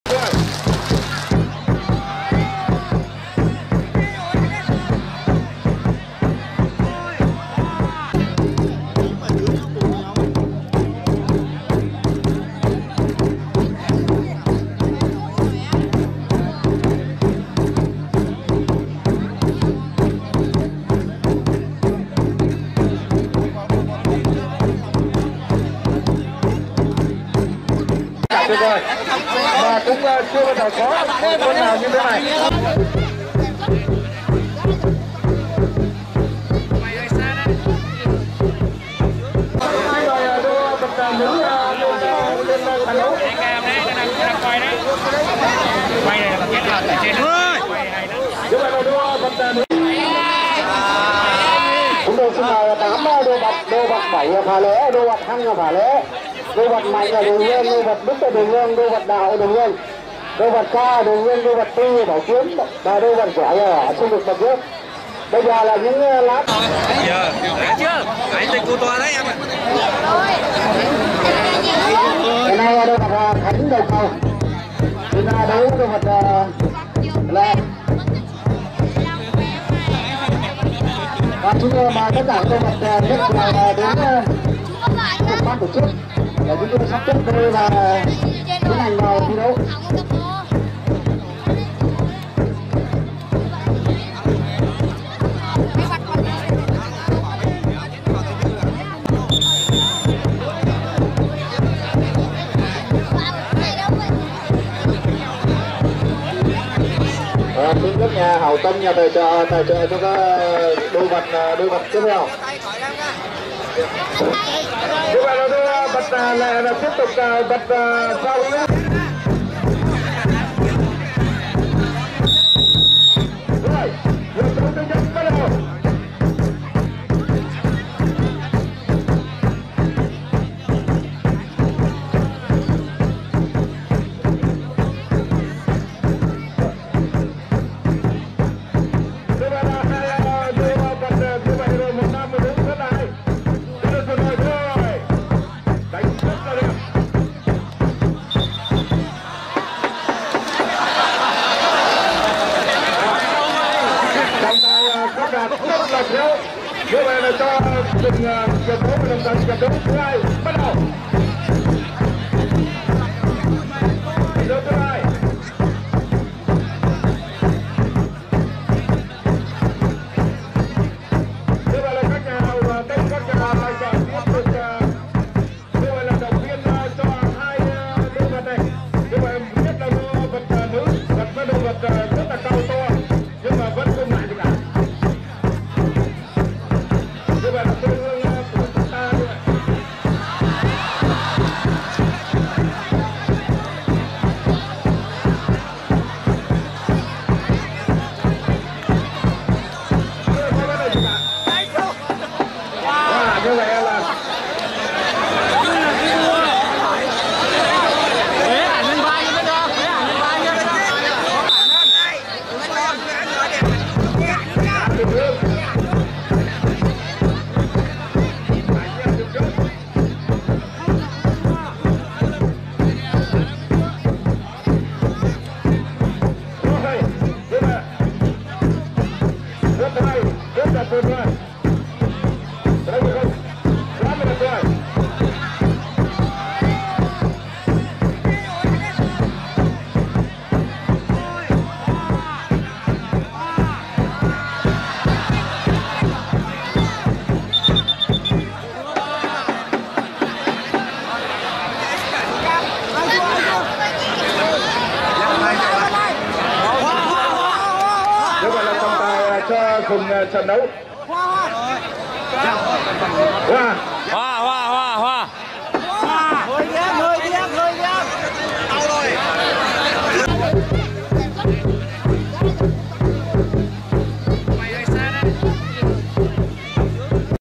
BÍ ẨN HÌNH XĂM KÍN CỦA ĐÔ VẬT NỮ
Đô vật nữ đã gây xôn xao trên cộng đồng mạng vừa qua. Tiếp theo là keo vật kinh điển của nữ chân dài đó.xin mời quý vị xem hết nhé.